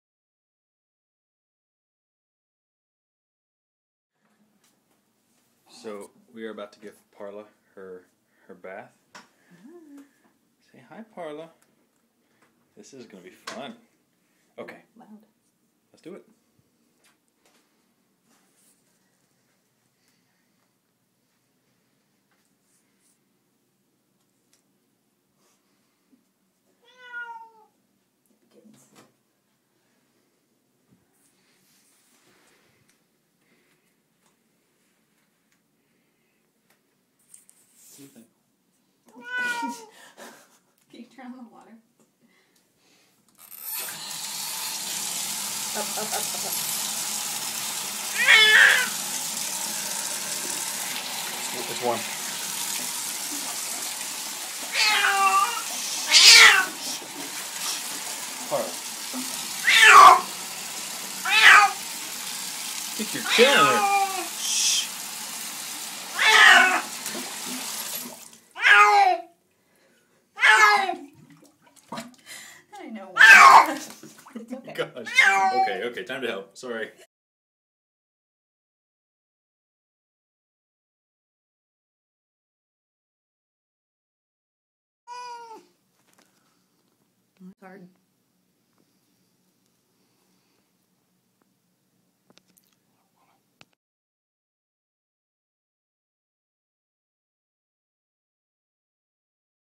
Loud Cat Gets a Bath
Black and white cat, Parla, gets a bath and is very very vocal about how much she hates the experience.  It leaves her quite traumatized.